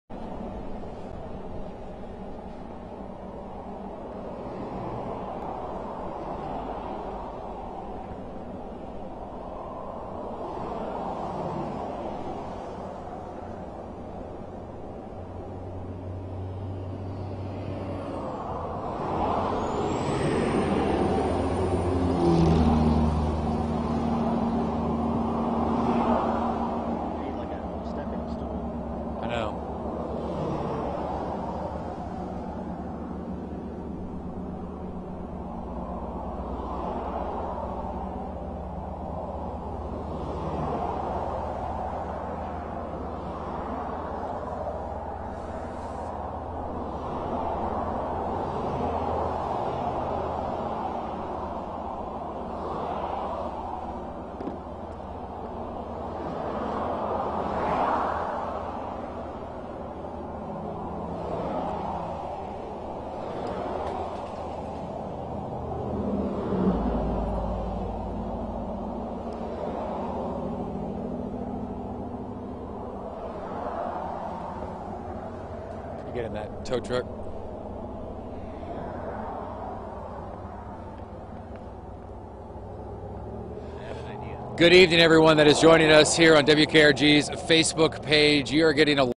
Multi-vehicle accident involving fuel spill closes I-65 NB lanes near exit 13
Saraland Police are working a multi-vehicle accident on the northbound lanes of I-65 just south of exit 13 at Highway 158 Wednesday evening.